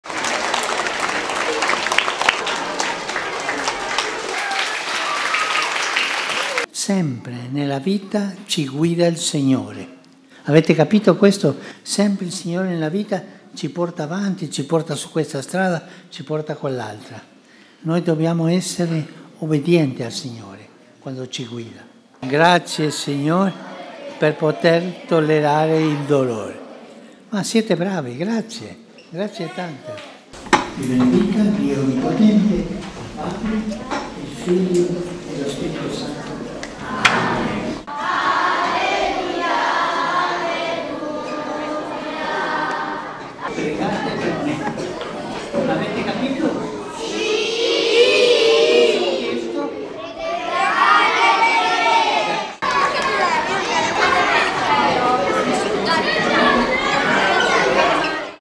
El Papa Francisco visitó “por sorpresa” a 200 niños en una parroquia de Roma
El Papa Francisco ha realizado una especial “escapada” en la tarde de este jueves 11 de abril y ha salido del Vaticano para encontrarse con un grupo de 200 niños en una parroquia de Roma.